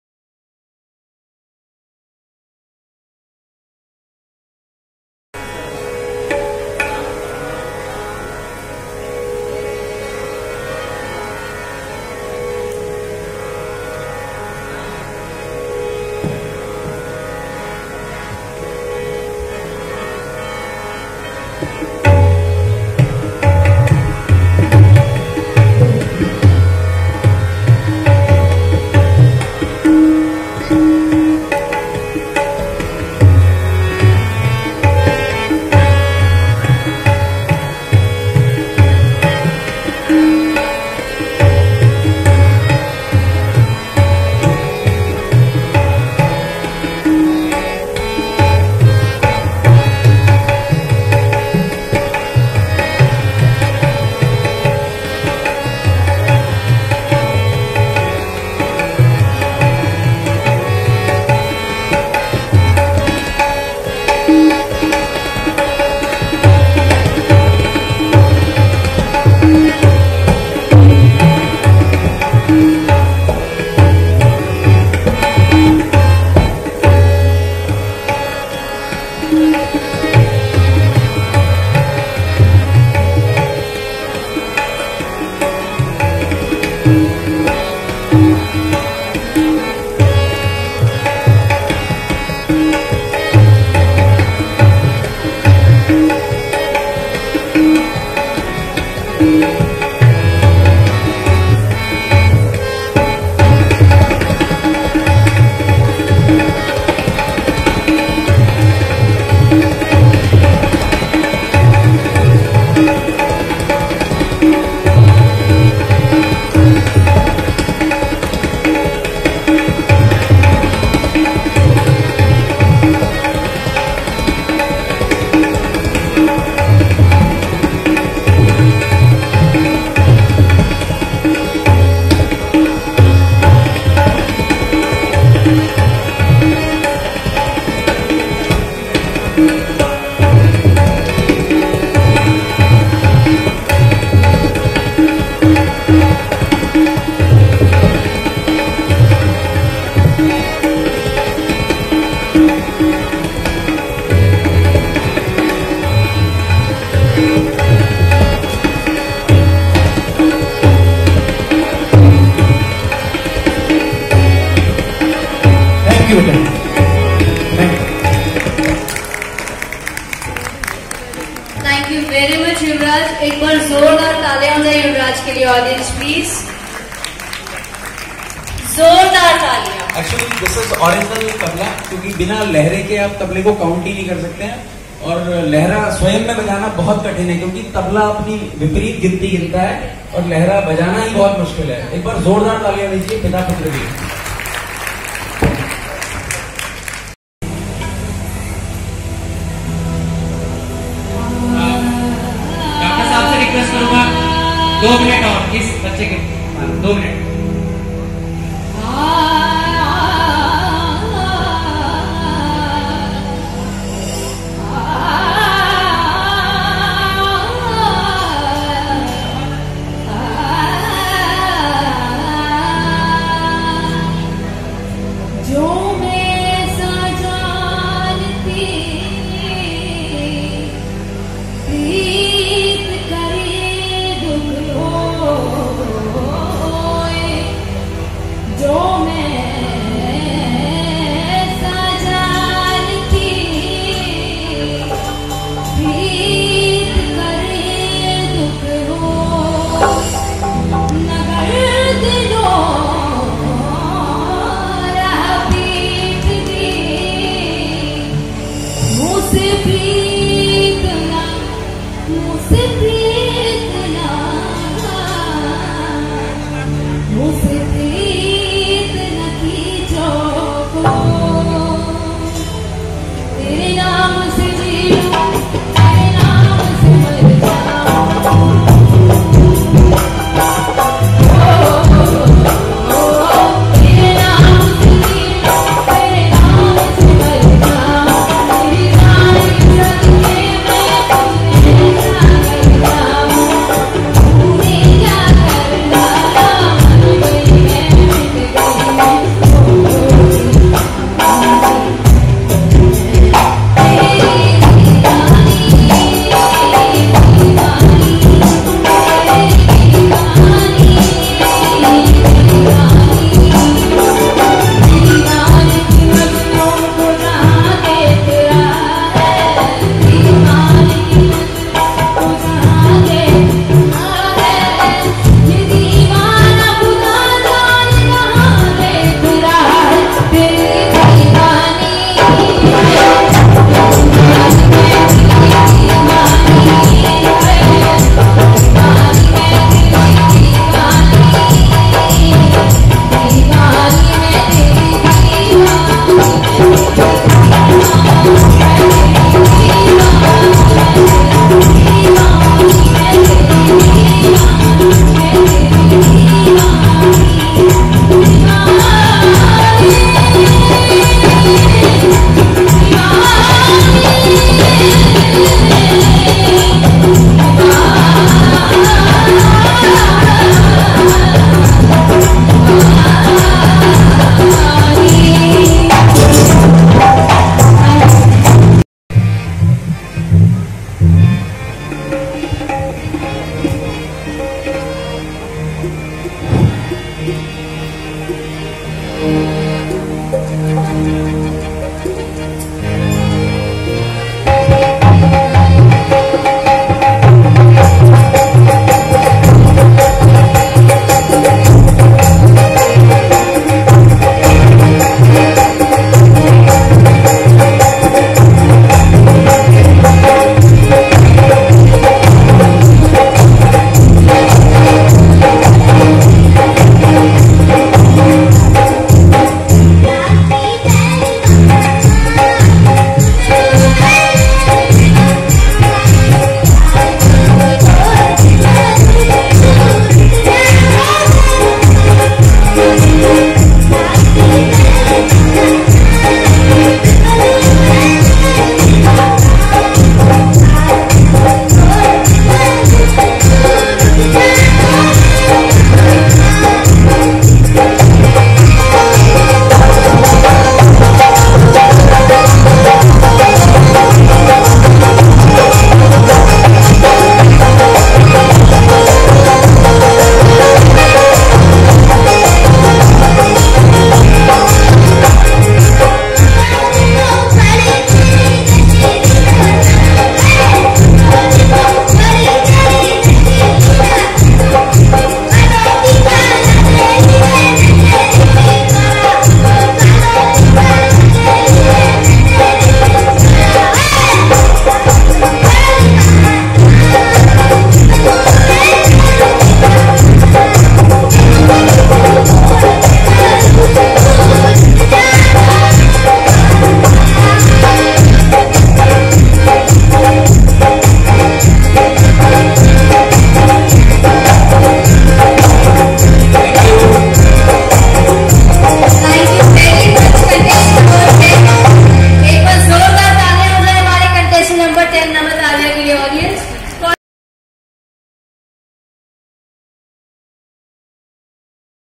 Supertalent 2019 Grand Finale II Uttranchal Deep II
This super talent Competition was conducted by Uttranchal Deep Haldwani, on 01 Sep 2019 at Pranay Vatica Hall Transport Nagar Haldwani. Mayor Haldwani Dr.Jogendra Rautela and Dr. J.S. Khurana was the chief guest.